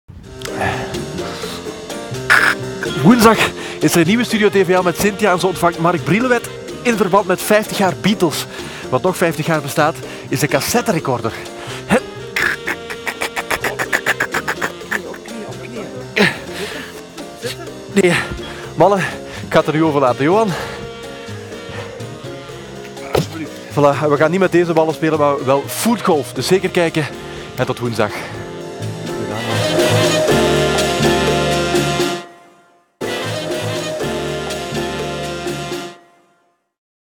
Stenen Ballen
Lukt het Maarten of niet de betonnen bal in de trailer op te tillen?